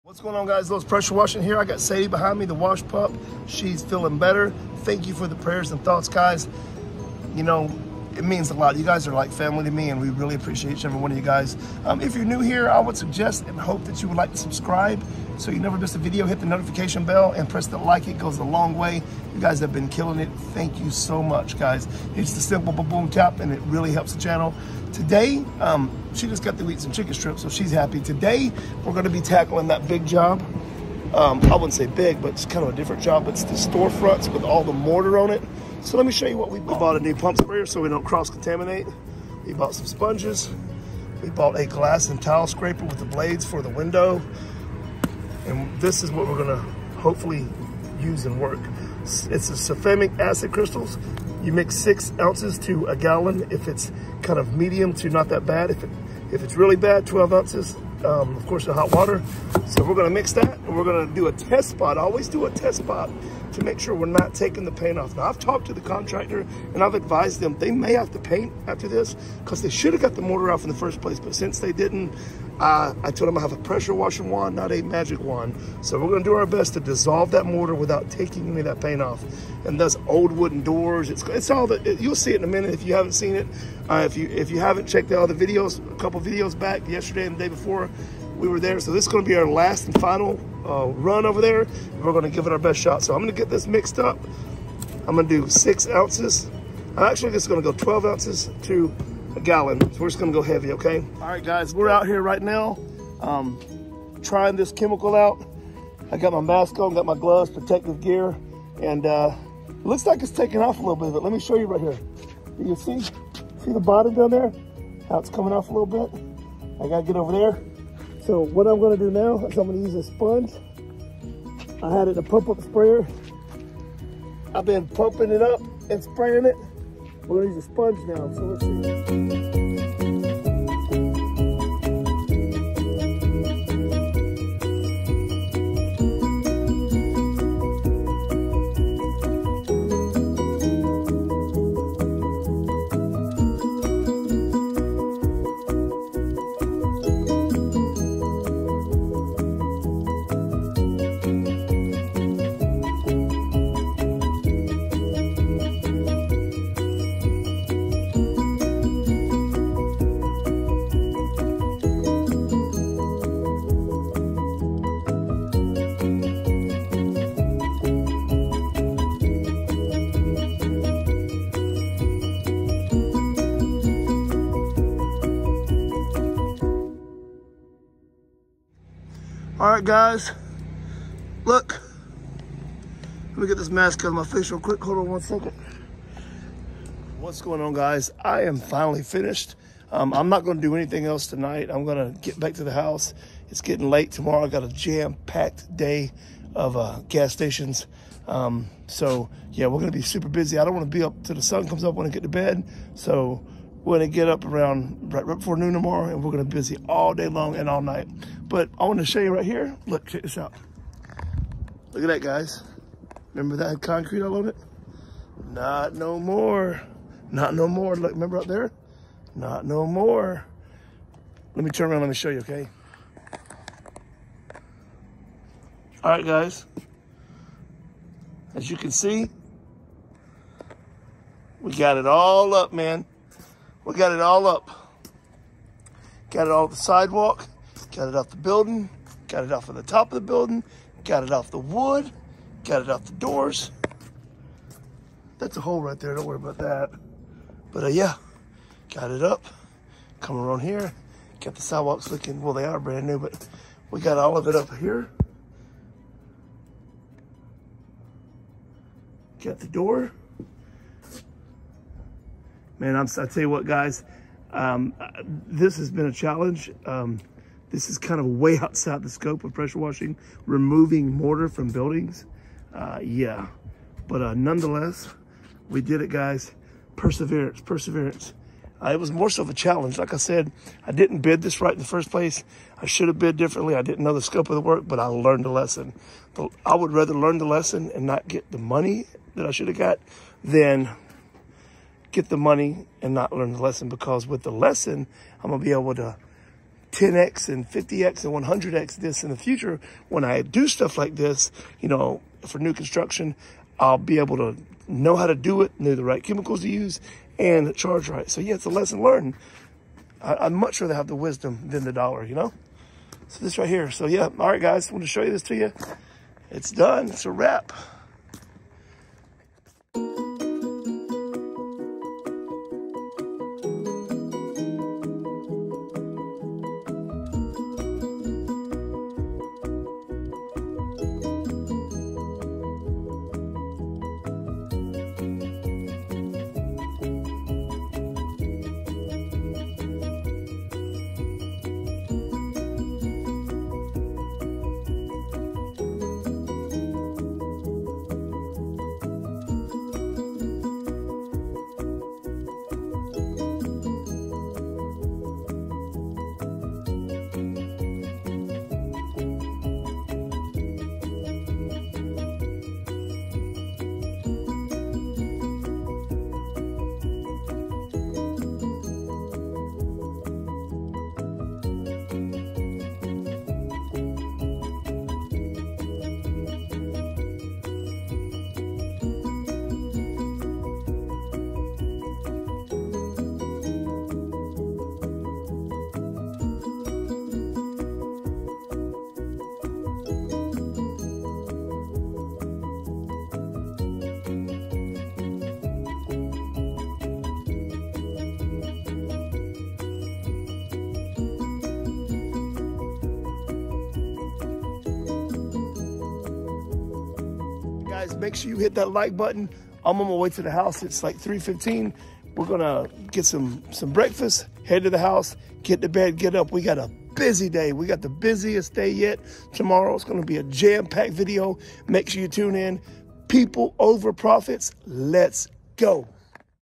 HOW TO PRESSURE WASH COMMERCIAL PROPERTY! HOW TO REMOVE CONCRETE,MORTAR FROM A ANY TYPE OF SURFACE!
HOW TO PRESSURE WASH COMMERCIAL PROPERTY! HOW TO CLEAN UP AND REMOVE CONCRETE/MORTAR/FROM EXTERIORS!